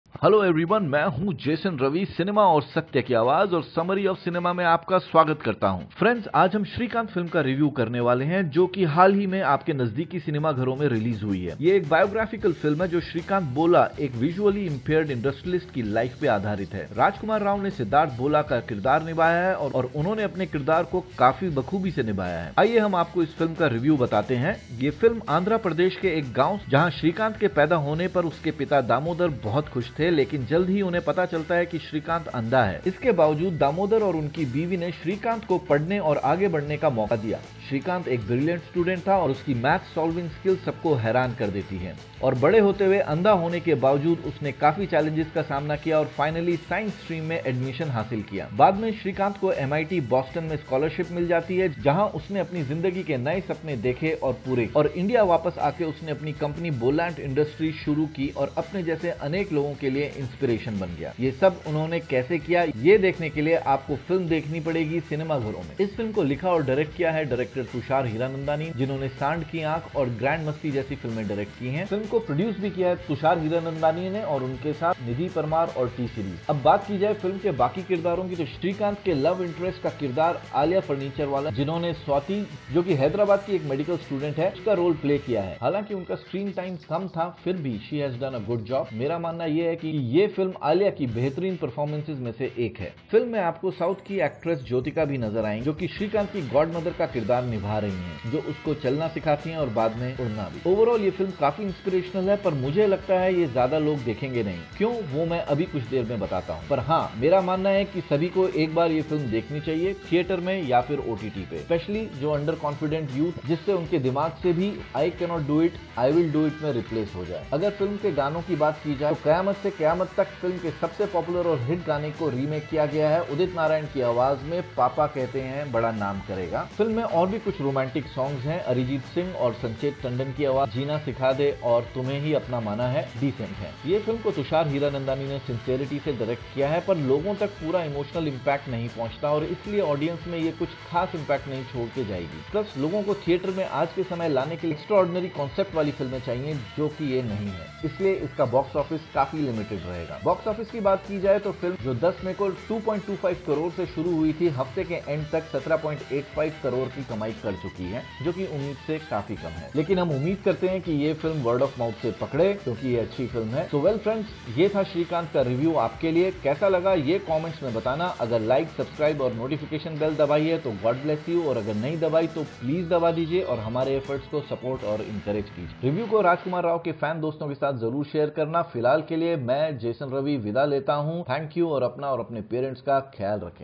Srikant Movie Review हिंदी
Srikanth Movie Review By Summary of Cinema. Srikanth Featuring Rajkumar Rao is based on Real Life of Srikanth Bolla who challenged every limitations of a Visually Impaired Man with Admission in Top American University to become the Business Partner of Former President APJ Abdul Kamal, An Impossible Story with a satisfactory Ending- Don't Miss this if you love motivational movies. Srikanth Full Movie Story Outline Explained & Positives Negatives would be shared in this Video. The Comeback of Bollywood or South Cinema is the only saviour with Kalki & Pushpa?